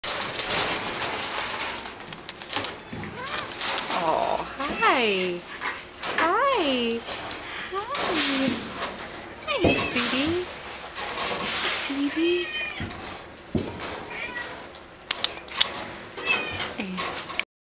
captian morgan n Jack D
Captain Morgan and Jack Daniels are adorable brothers that were surrendered together. Please come visit them at JTAC